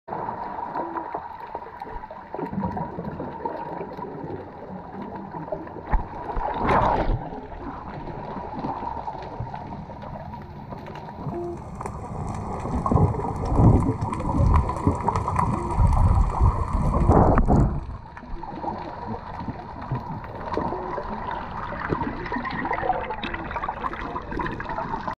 Underwater Statues Destin, FL
Underwater statues on the East Jetties (specifically the Finger Jetty as known by locals) in Destin, FL